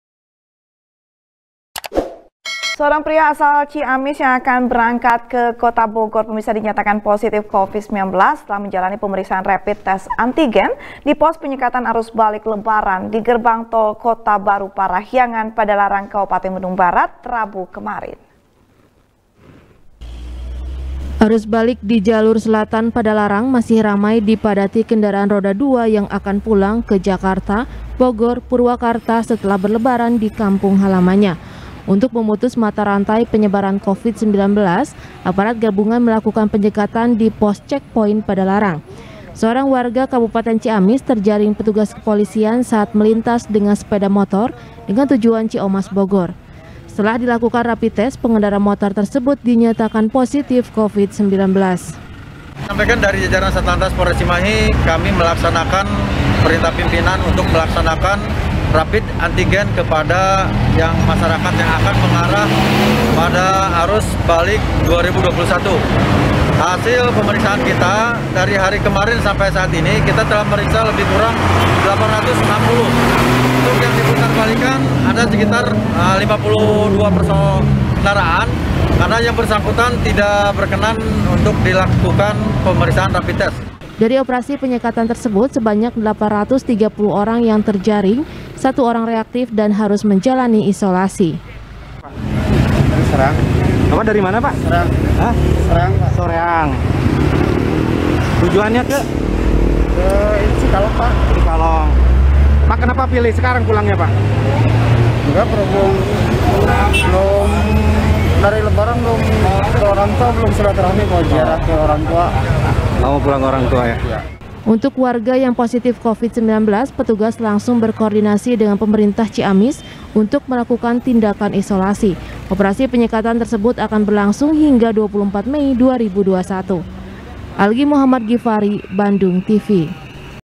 Arus Balik, Satu Warga Terjaring Positif Covid-19 | SBR | BANDUNGTV
SBR | BANDUNGTV | FARHAT MUMTAZ | Seorang pria asal Ciamis yang akan berangkat ke Bogor, dinyatakan positif Covid-19 setelah menjalani pemeriksaan rapid test antigen,  di pos penyekatan arus balik Lebaran, di Gerbang Tol Kota Baru Parahyangan Padalarang Kabupaten Bandung Barat, Rabu kemarin.